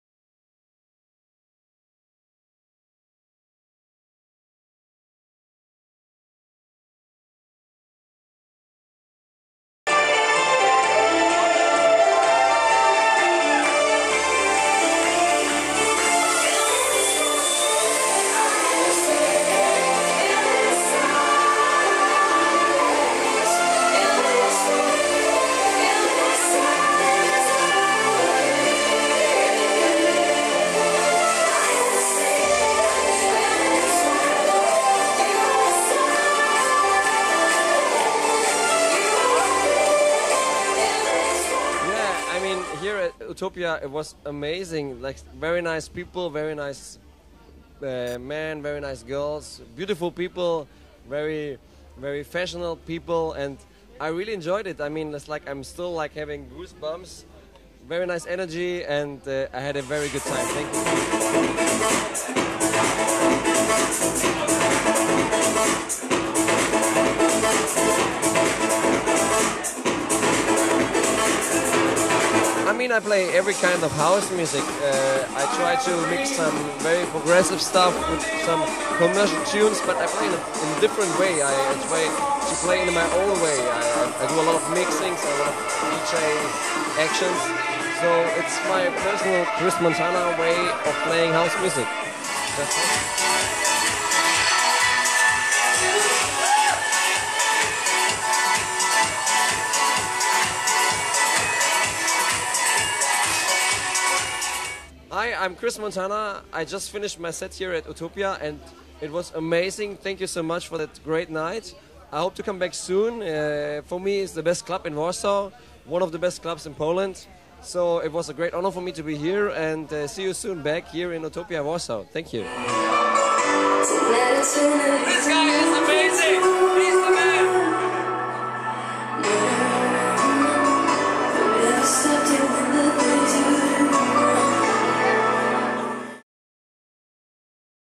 CHRIS MONTANA
Po bardzo długiej przerwie Utopia wznawia cykl swoich najważniejszych imprez -- Outsider. W ramach tego cyklu w Utopii gościliśmy największe nazwiska muzyki house, m.in. Antoine Clamaran, Axwell, Bob Sinclar, David Guetta, Junior Vasquez, Martin Solveig.
Już 30 czerwca do tej listy, jako numer 51 dołączy Chris Montana -- znakomity producent i DJ. Uwielbiany przez klubowiczów m.in. Ibizy, Mykonos, współpracujący z największymi gwiazdami sceny klubowej.
VIDEO: ŁUKASZ DOBROWOLSKI